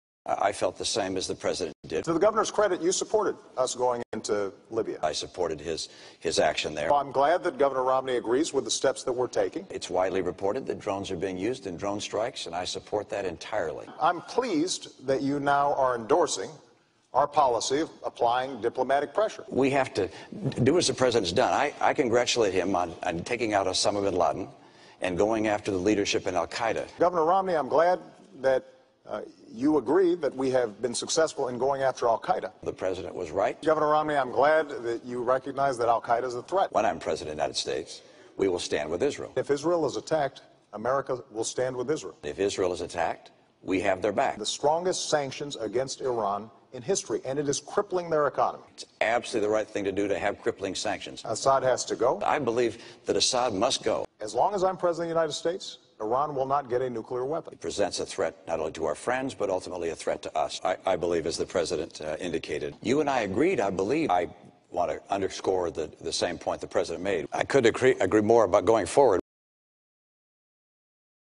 Romney: The Flip-Flopping Political Weasel
If Romney was half as hard as he was on Trump, maybe, just maybe he would of made some headway towards the presidency.

But you didn't so dry up and blow away Mitt.